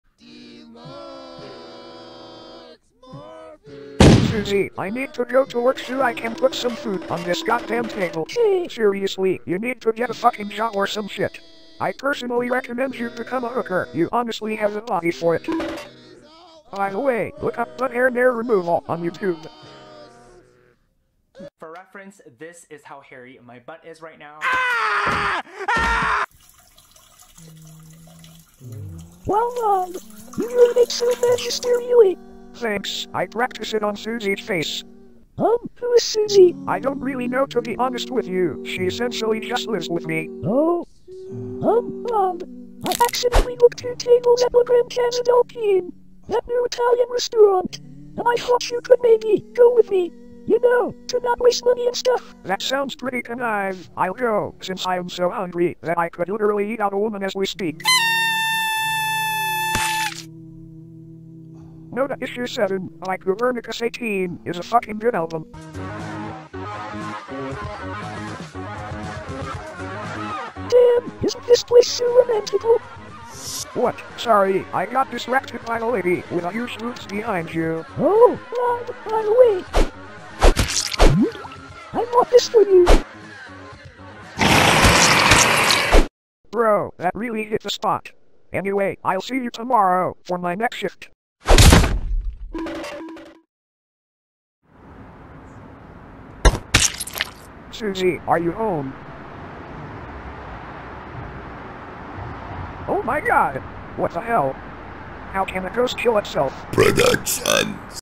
Bob takes Ella on a date (My Ghost friend parody)
Parody of My Ghost Friend by dsand00

Was supposed to finish this before the 100th episode
You can watch the series here, it’s pretty charming